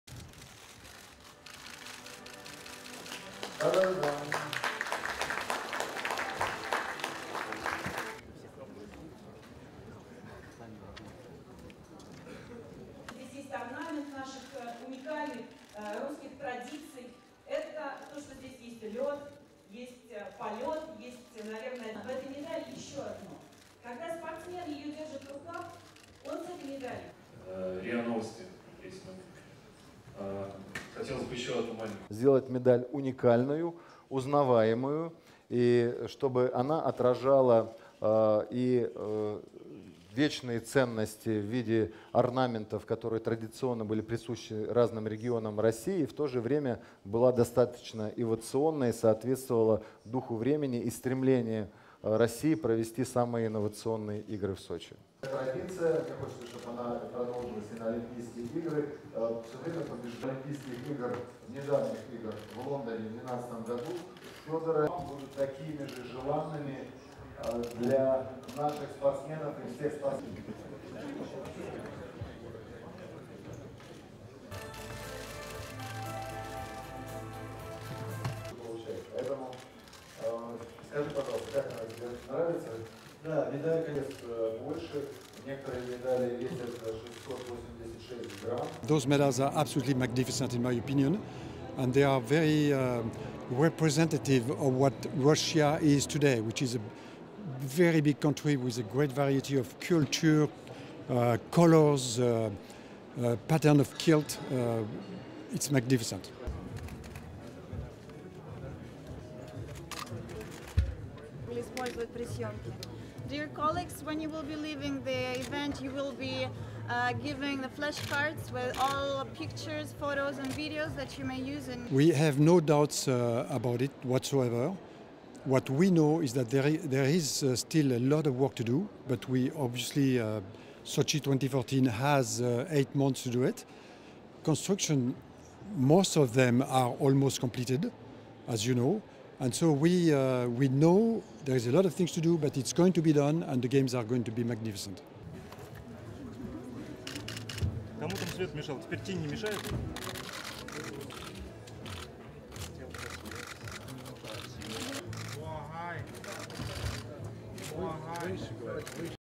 Organisers present "patchwork quilt" design of medals for 2014 Games
AP TELEVISION
1. Presentation of medals for 2014 Winter Olympic Games in Sochi, zoom in as Russian Olympic champion Svetlana Zhurova lifts out gold medal to show to media
2. Media at event
3. Various of Zhurova showing Olympic gold medal to media
4. Wide of Dmitry Chernyshenko, President of Sochi 2014 Olympic Organising Committee, and Russian Deputy Prime Minister Dmitry Kozak on stage
5. SOUNDBITE: (Russian) Dmitry Chernyshenko, President of Sochi 2014 Olympic Organising Committee:
"(The aim was) to make a unique and memorable medal that would reflect the eternal values that are represented by traditional motifs relating to different regions of Russia, and at the same time to create somthing that would be suitably innovative and correspond with the zeitgeist and correspond to Russia's aspiration to hold the most innovative games in Sochi."
6. Various of gold, silver and bronze Olympic medals in a display case
7. Wide of man walking onto stage with box of Paralympic Games medals
8. Mid of cameras filming
9. Various of Russian Paralympic champion Fyodor Trikolich presenting Paralympic gold medal to media
10. SOUNDBITE: (English) Jean-Claude Killy, Chairman of the IOC Coordination Commission for Sochi 2014:
"Those medals are absolutely magnificent, in my opinion. And they are very representative of what Russia is today, which is very big country with a great variety of culture, colours, pattern of quilt. It is magnificent."
11. Various of Olympic medals in display case
12. SOUNDBITE: (English) Jean-Claude Killy, Chairman of the IOC Coordination Commission for Sochi 2014:
(answering question whether construction will be finished in time for the games)
"We have no doubts about it whatsoever. What we know is that there is still a lot of work to do, but obviously Sochi 2014 has eight months to do it. Construction, most of them, are almost completed, as you know. And so, we know there is a lot of things to do, but it's going to be done, and the Games are going to be magnificent."
13. Mid of photographers taking pictures of medals
14. Various of Olympic gold medal being presented
STORYLINE:
The medals that are to be awarded to athletes at the Sochi 2014 Olympic and Paralympic games were presented to the media on Thursday in Saint Petersburg.
The medals for the 2014 Winter games feature a "patchwork quilt" design representing the different regions of Russia.
Organisers produced a record 1,300 medals for the Olympics and Paralympics, which will be held in the Black Sea resort next February and March.
The medals depict the sun's rays reflecting through a prism of mountains and beaches.
The design was approved on Thursday by the IOC (International Olympic Committee) executive board.
Jean-Claude Killy, the chairman of the IOC Coordination Commission for Sochi 2014, described the medals as "magnificent".
Killy acknowledged there was still much to be done to prepare for the Games but he was certain that the sites would be ready in time.
The medals are 0.4 inches (one centimetre) thick and 4 inches (10 centimetres) in diameter.
The Olympic gold medals weigh 16 ounces (0.45 kilograms).